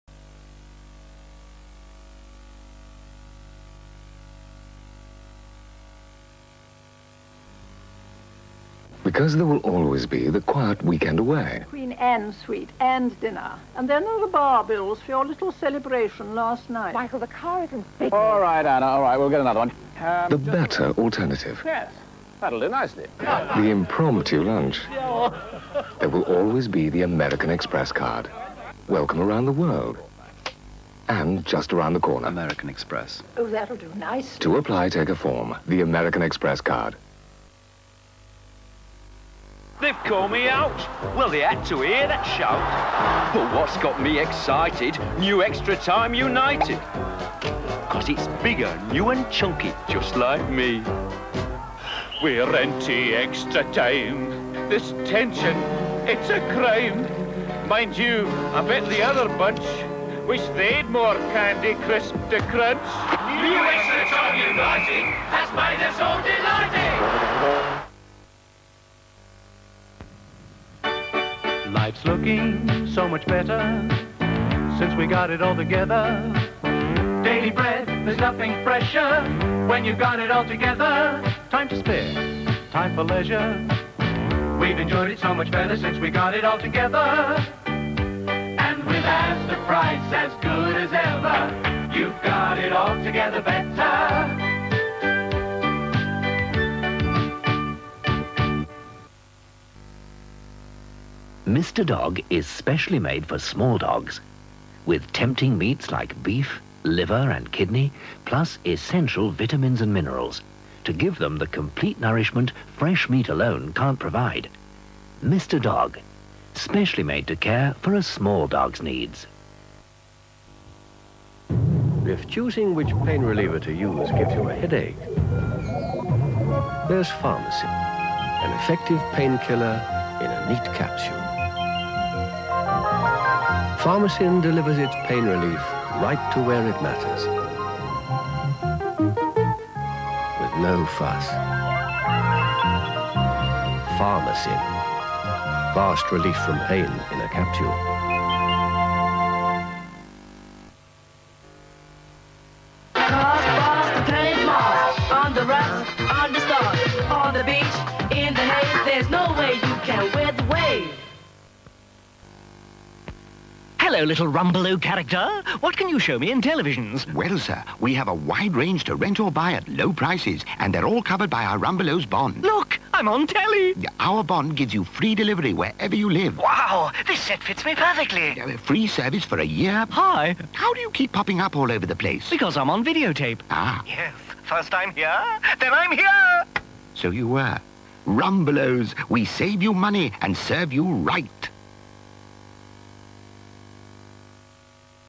ITV | Adverts | 1982
21st October 1982 - Adverts from an unknown region during the film Gold